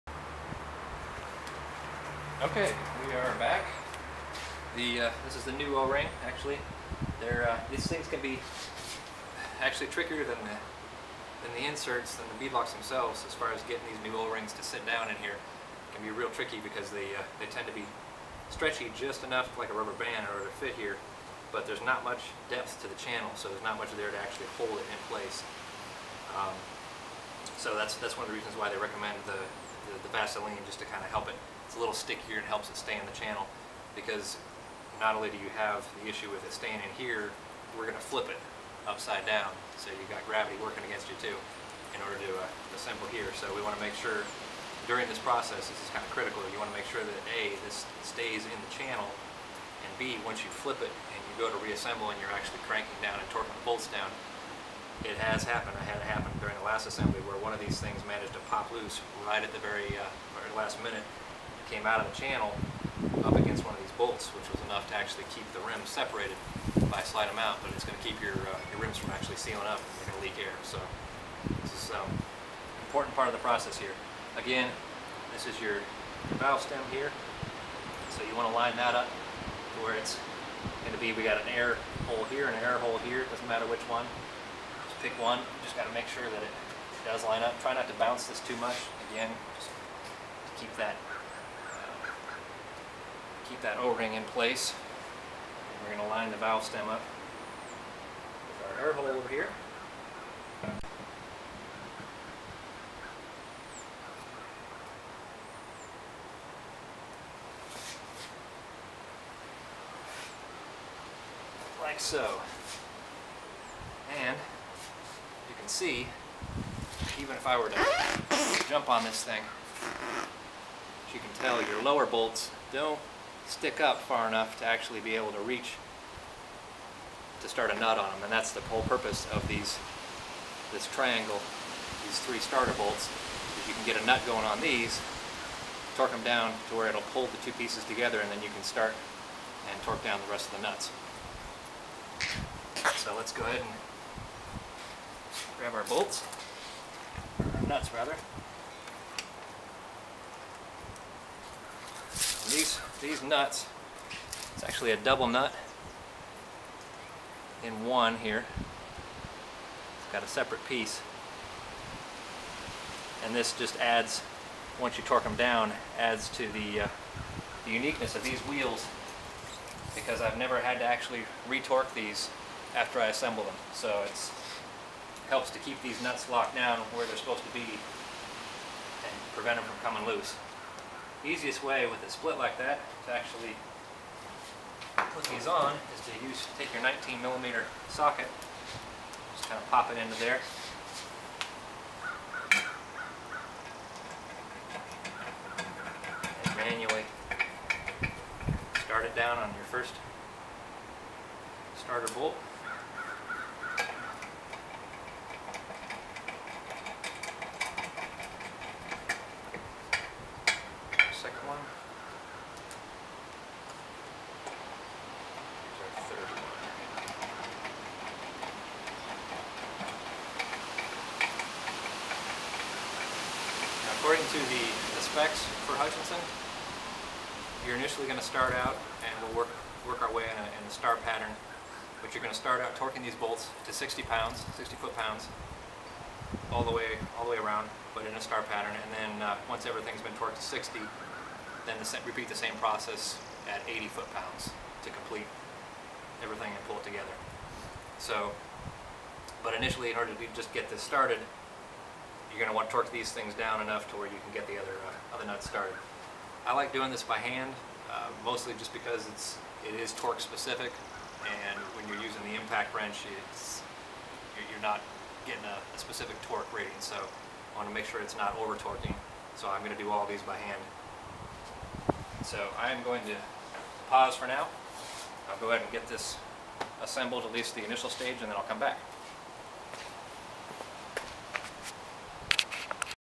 Expeditionr Presents: Hutchinson Rock Monster Assembly - Part 3
Expeditionr presents part three in a series that explains how to properly and safely assemble Hutchinson's Rock Monster internal beadlock wheels.